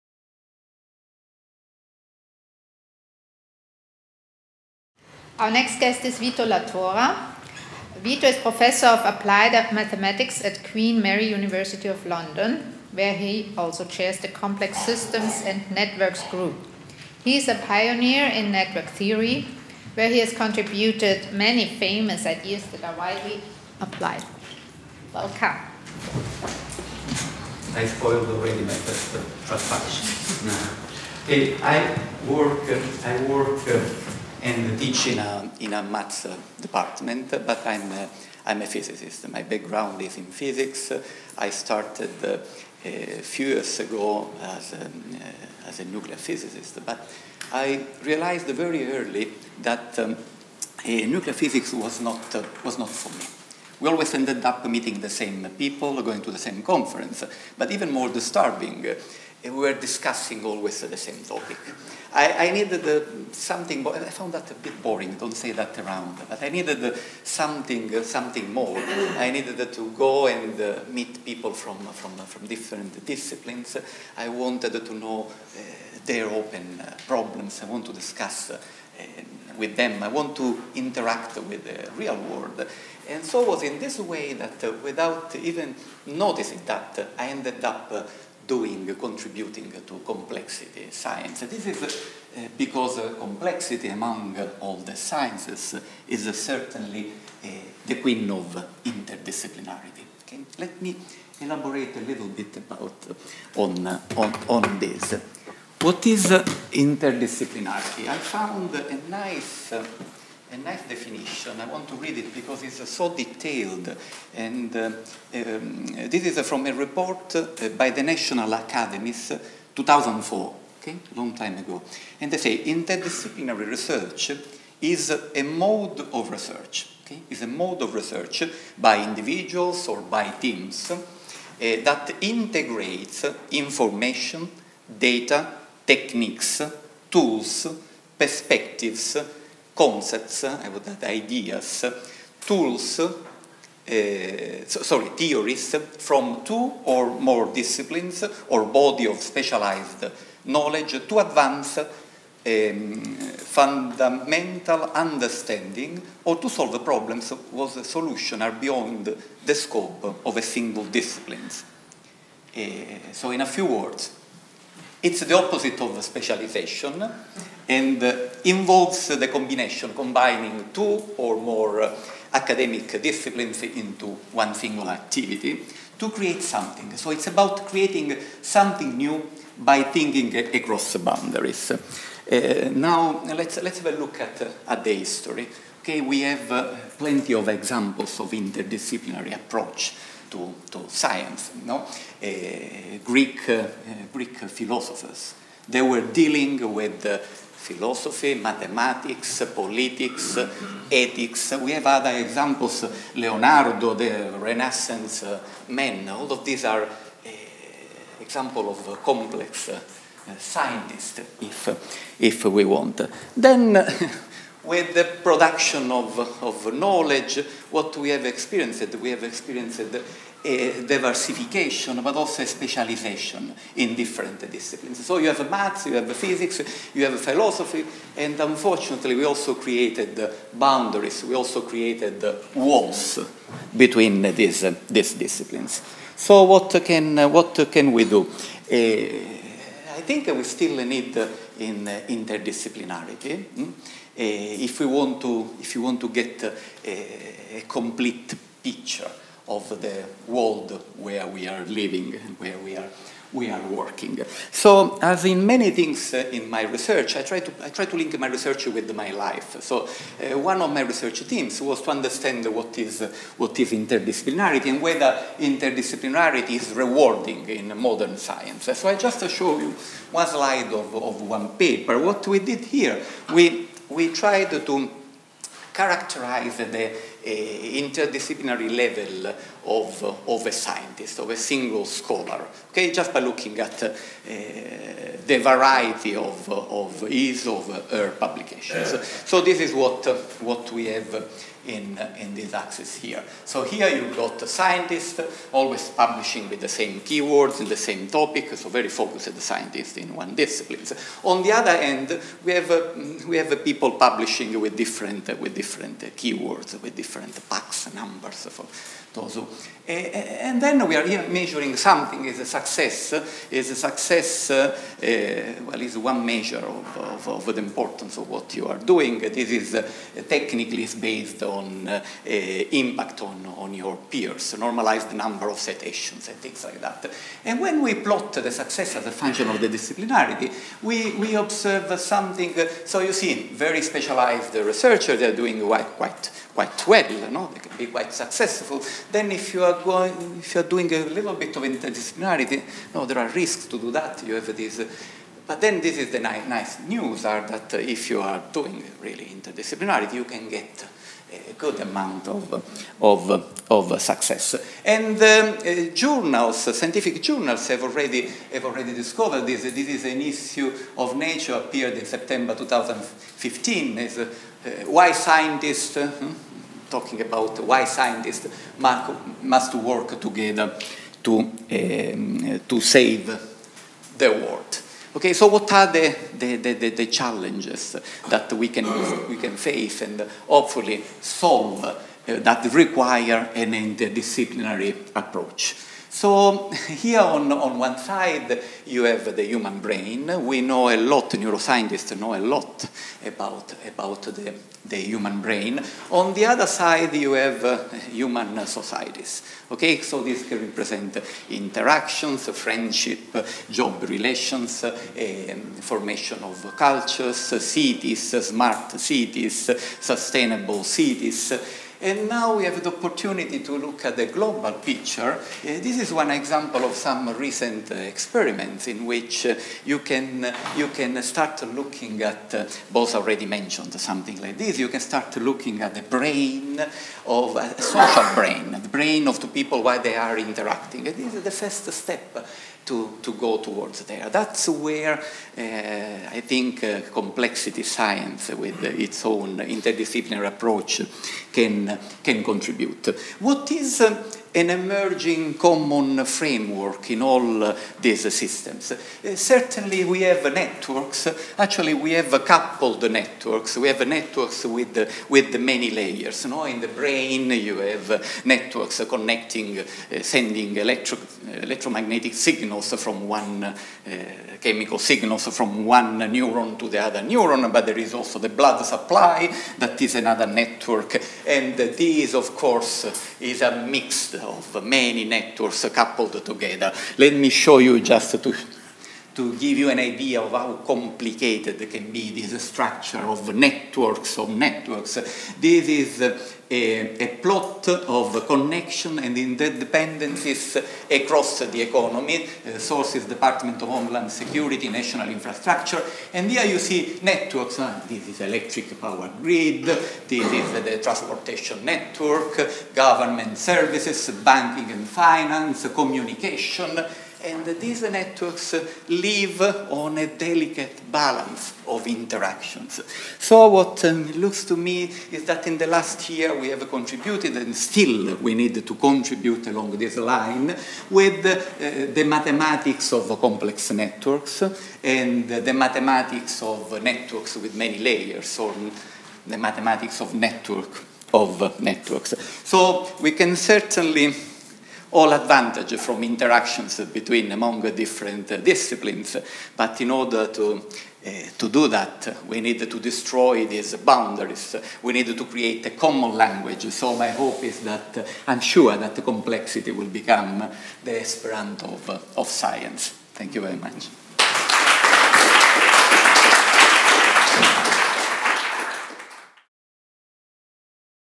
Vito Latora @ CSH Vienna | "Complexity as Esperanto of science"
Interdisciplinarity is needed to get a complete picture of the world in which we are living and working, says Vito Latora. To be successful with it, researchers have to destroy boundaries and have to create a common language.